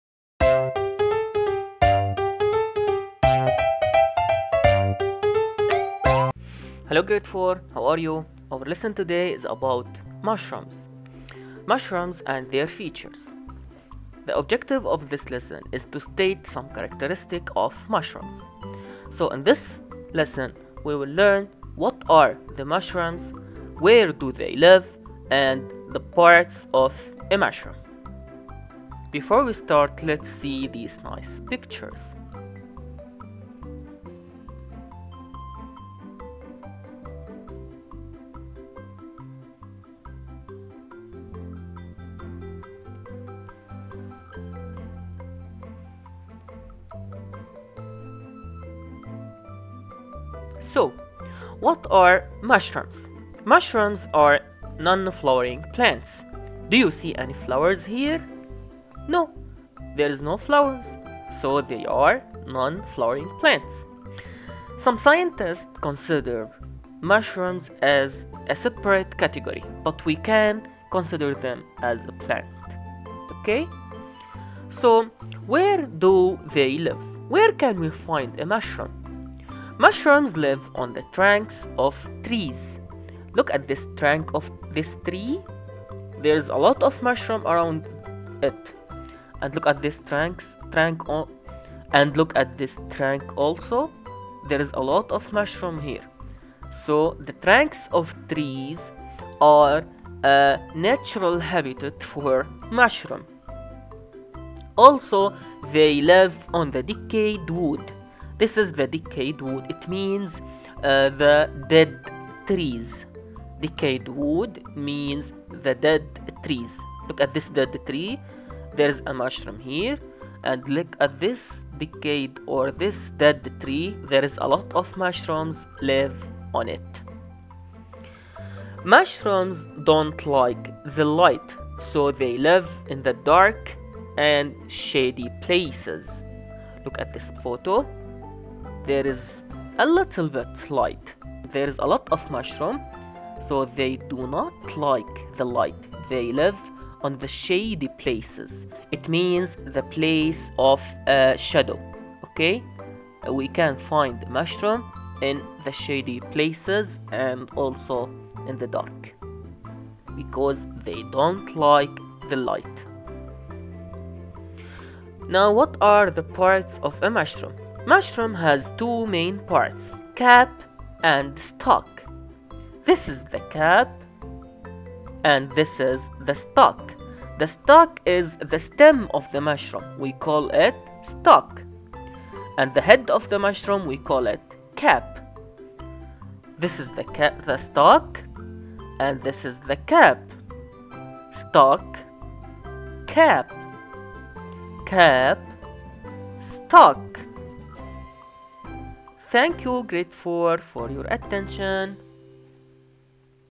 Mushrooms and their characteristics
this video state the general characteristics of mushrooms, what they are? where do they live? parts of mushrooms.